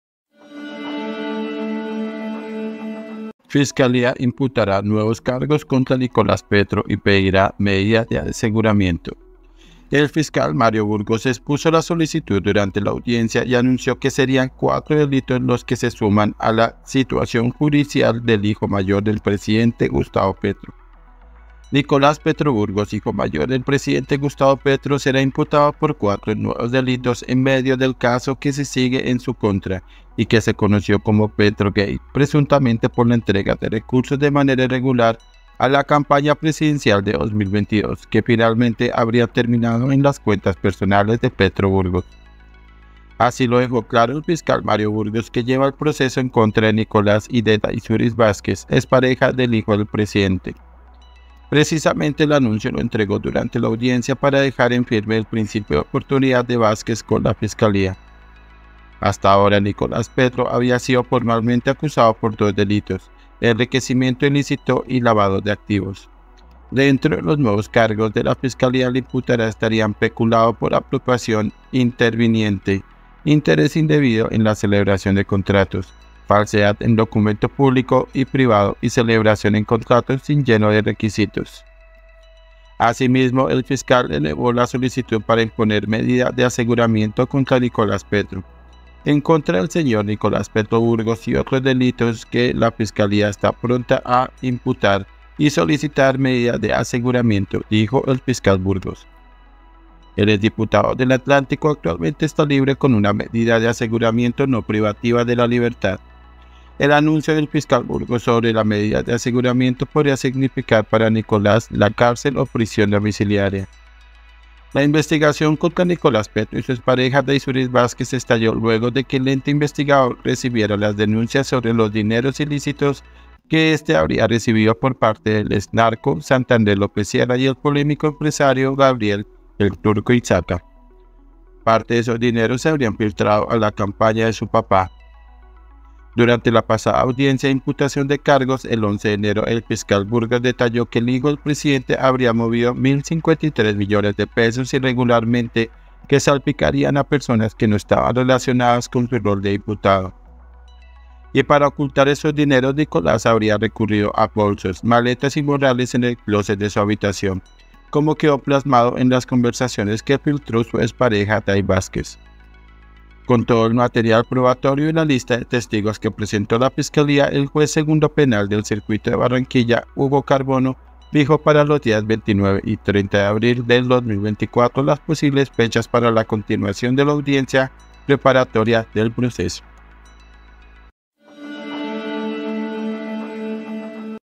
ÚLTIMA HORA ⛔ NUEVOS CARGOS CONTRA NICOLAS PETRO Y POSIBLE MEDIDA DE ASEGURAMIENTO
Fiscalía imputará nuevos cargos contra Nicolás Petro y pedirá medida de aseguramiento. El fiscal Mario Burgos expuso la solicitud durante la audiencia y anunció que serían cuatro delitos los que se suman a la situación judicial del hijo mayor del Presidente Gustavo Petro.
EL COLOMBIANO